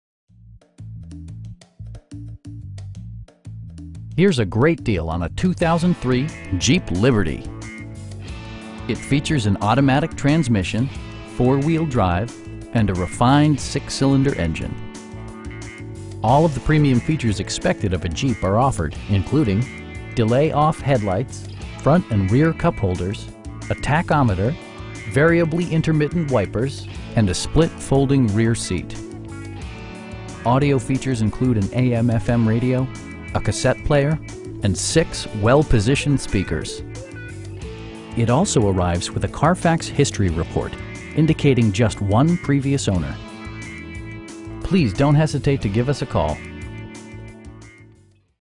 2003 Jeep Liberty 4dr Sport 4WD in Hooksett, NH 03106-6415
Merchants Auto

Here's a great deal on a 2003 Jeep Liberty. It features an automatic transmission, 4-wheel drive, and a refined 6 cylinder engine. All of the premium features expected of a Jeep are offered, including: delay-off headlights, front and rear cupholders, a tachometer, variably intermittent wipers, front bucket seats, and a split folding rear seat. Audio features include an AM/FM radio, a cassette player, and 6 well positioned speakers. It also arrives with a Carfax history report, indicating just one previous owner. We pride ourselves on providing excellent customer service. Please don't hesitate to give us a call.